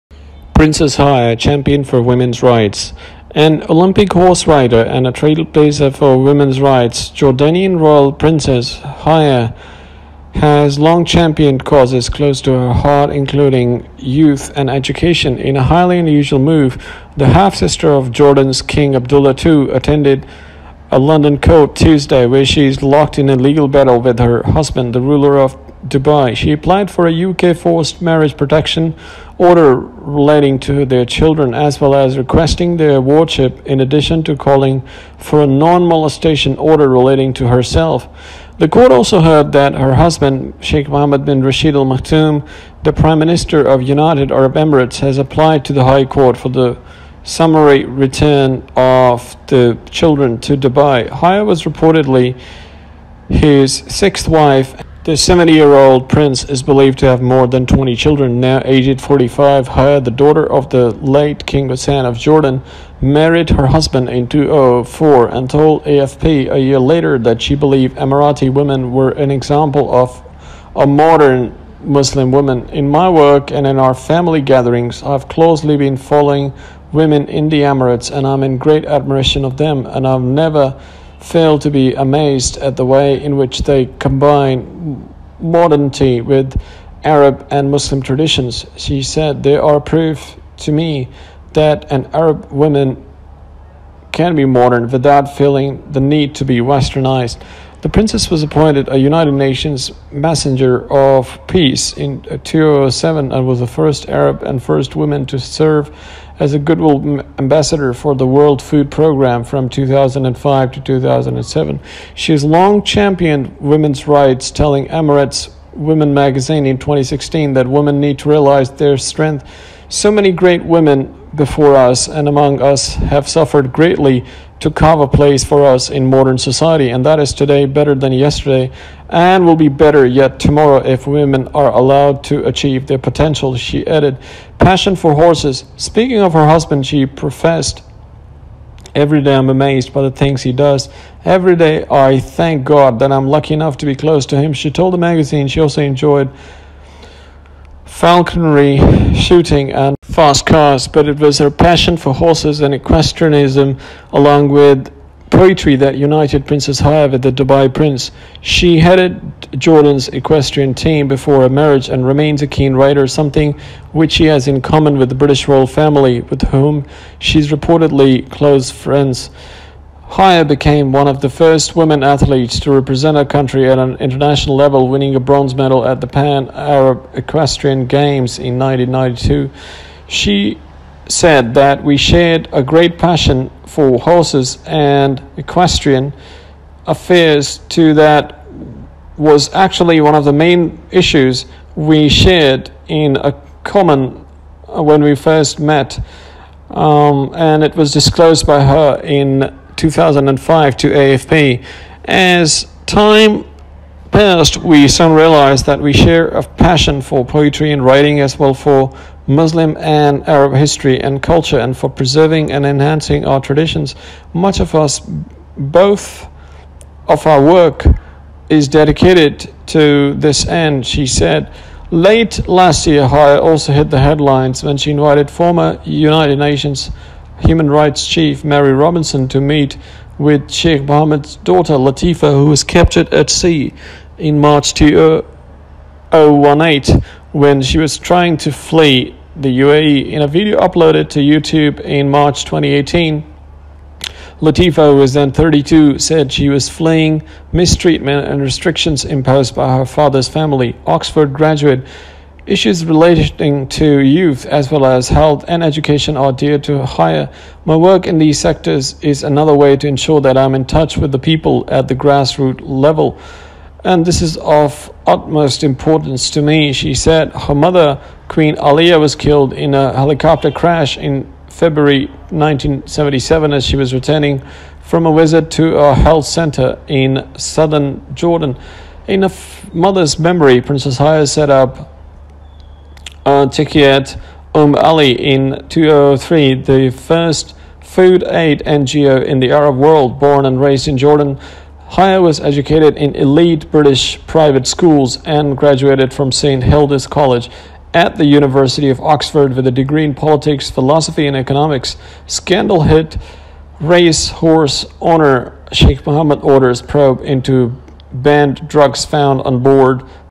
Princess Haya Wife Sheikh Mohammed bin Rashid Al Maktoum: champion for women's rights.
An Olympic horse rider and a trailblazer for women's rights, Jordanian royal Princess Haya has long championed causes close to her heart including youth and education.